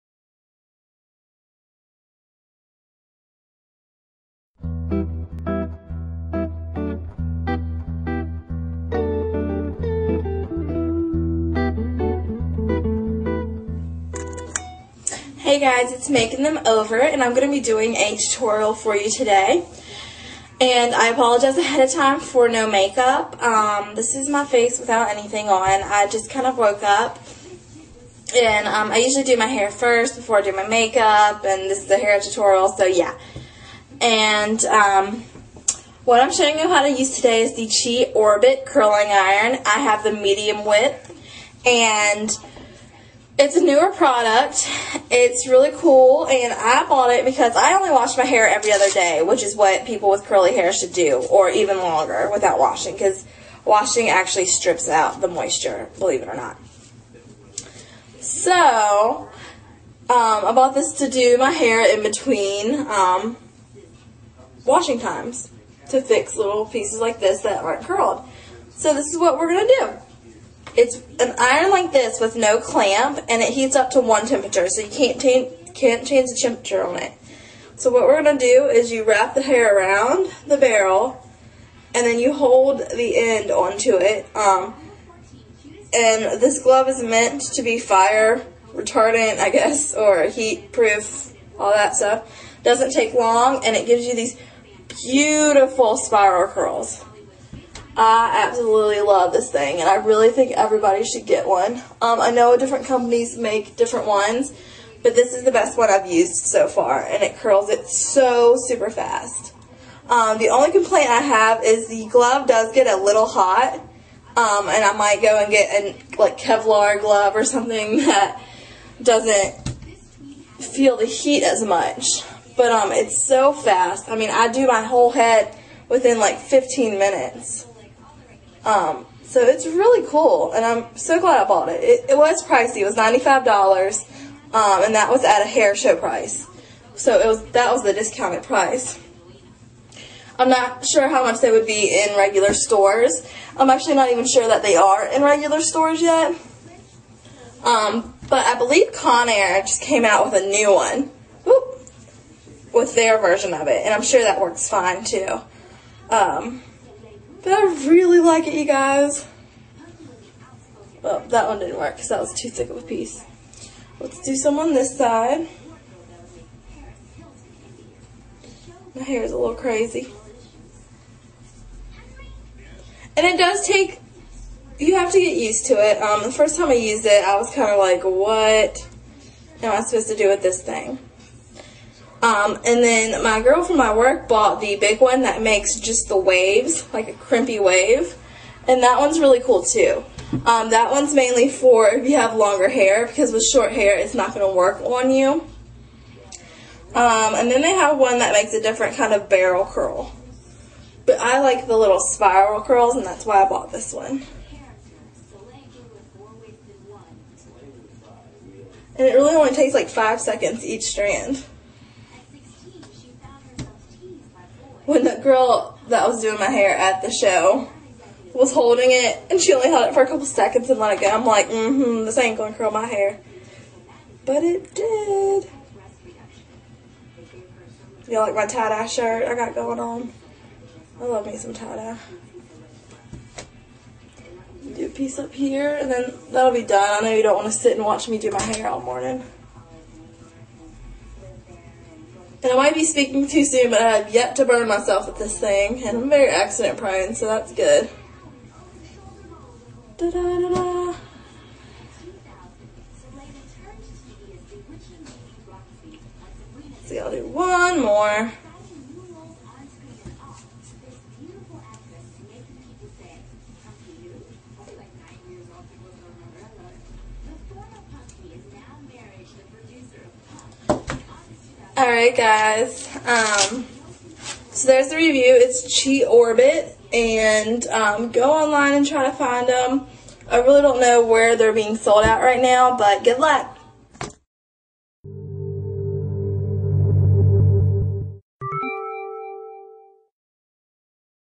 Review Tutorial Chi Orbit
this is me showing you how to use the new Chi Orbit curling iron